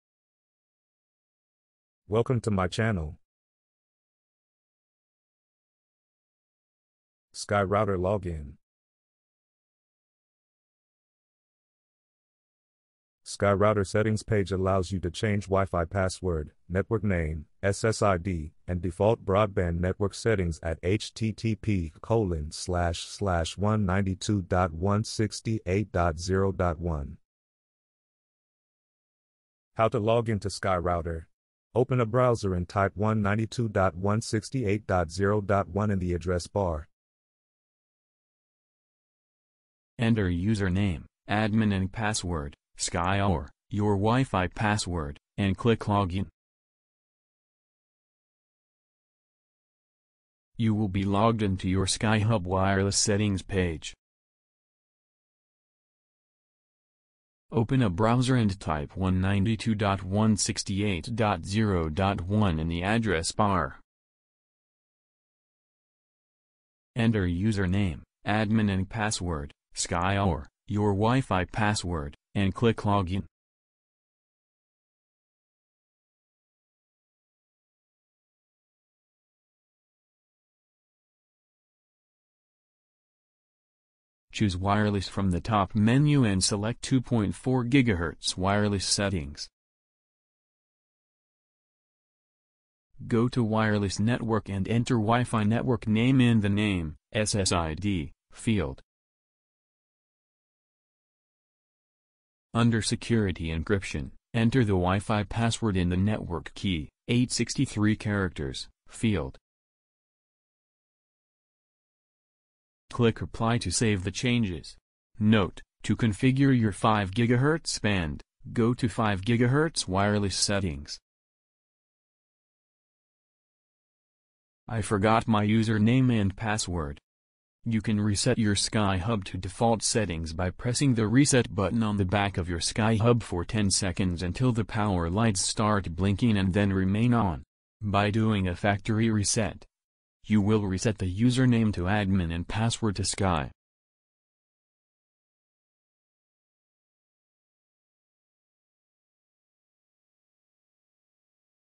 Sky Router Login
If you’re the kind of person who thrives on full control, buckle up, because I’m going to show you how to march into your Sky router settings and make it a bespoke networking tool! From your WiFi password and Network Name (SSID), to your broadband network settings; all are within your grasp at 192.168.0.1. Whether you’re a seasoned network wizard or a rookie looking for a router walkthrough, there’s something here for you. Stick around, and let’s customize your cyber universe together!